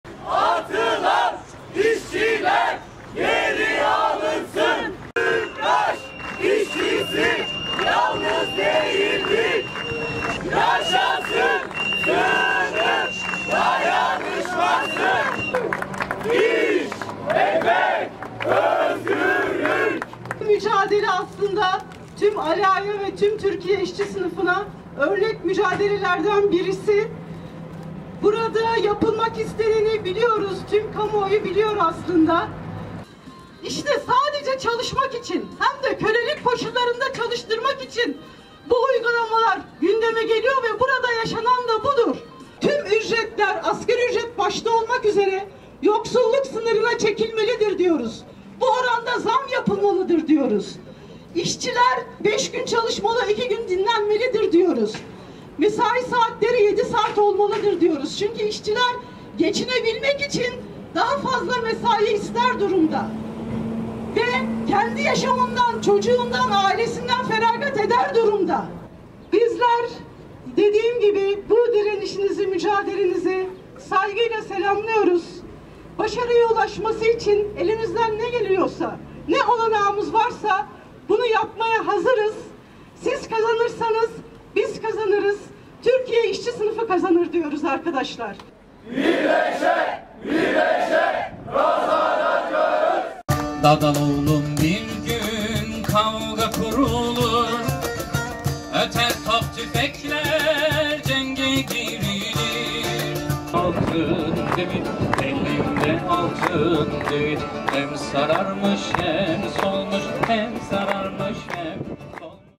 TÜPRAŞ işçilerini ziyaret eden Emek Partisi, "İşten atmalar yasaklansın!" dedi
Koç Holding'in TÜPRAŞ Aliağa rafinerisinde 22 işçinin iş akdini feshetmesinin ardından işçilerin eylemleri devam ediyor. Eylemlerinin altıncı günde işçileri Emek Partisi (EMEP) İzmir İl Örgütü ziyaret etti.

“TÜPRAŞ işçisi yalnız değildir, işten atmalara karşı birlik, mücadele, dayanışma” çağrısı yapan pankart ile alana giren EMEP üyelerini, işçiler alkışlar ve sloganlarla karşıladı.

Sık sık “TÜPRAŞ işçisi yalnız değildir”, “İş, ekmek, özgürlük”, “İşçilerin birliği sermayeyi yenecek”, “Atılan işçiler geri alınsın”, “Yaşasın sınıf dayanışması” sloganları atıldı.

Ziyarette konuşan EMEP İzmir İl Başkanı Emine Uyar, “Aliağa TÜPRAŞ işçileri olarak, ‘İşçi kıyımında pazarlık olmaz’ diyerek, mücadele yolunu tercih ederek, işyerini terk etmeme eylemiyle, çıkarılmak istenen arkadaşlarınıza ve aslında kendi örgütlülüğünüze ve geleceğinize sahip çıktınız” dedi.